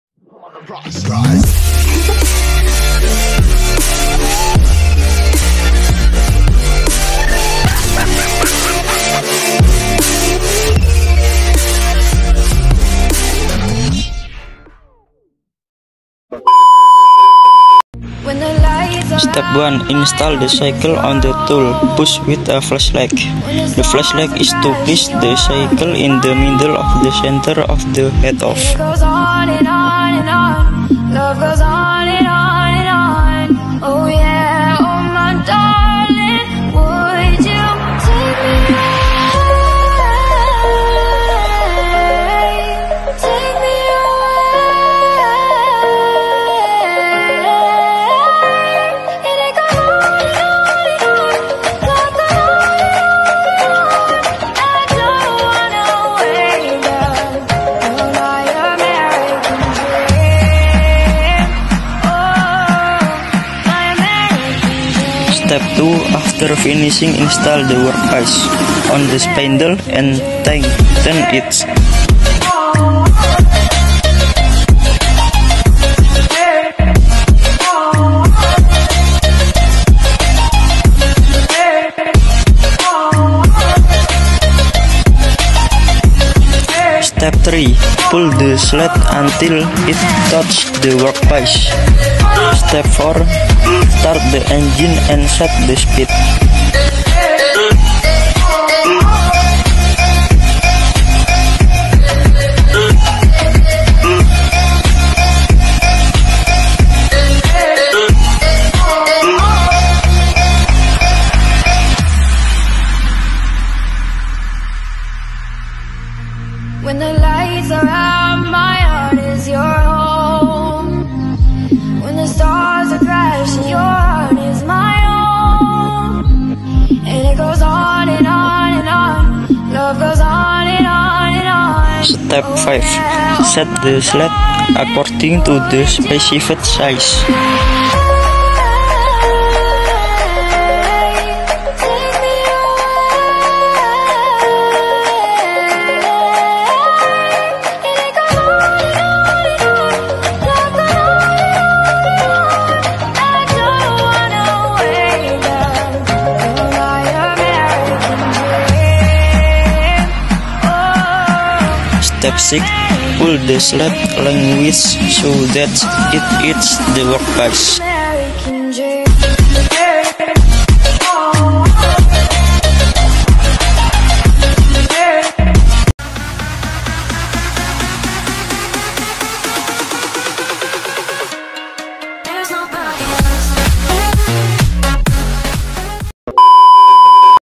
BELAJAR MENGOPERASIKAN MESIN BUBUT!!!! DALAM BAHASA INGGRIS#bhsinggris
pertama tama kita pasang pahat lalu kita ukur dengan jari kita minimal dua jari jari telunjuk dan jari tengah, setelah itu kita senterkan pahat sama kepala lepas biar membubut kita rapi dan tidak ada kecelakaan benda kerja atau (rusak), lalu kita pasang benda kerja dan jangan sampai benda kerja itu oleng atau goyang-goyang dan lalu biar gampang kita bisa pakek kepala lepas untuk menahan atau biar tidak oleng, setelah itu dan atur kecepatan yang sesuai dengan keinginan lalu kita nyalakan mesin dan kita membubut benda kerja sesuai apa yang sudah di buat dengan gambar job (benda kerja yang sudah di buat melalui gambar).

sekian dari saya wassalamu'alaikum wr. wb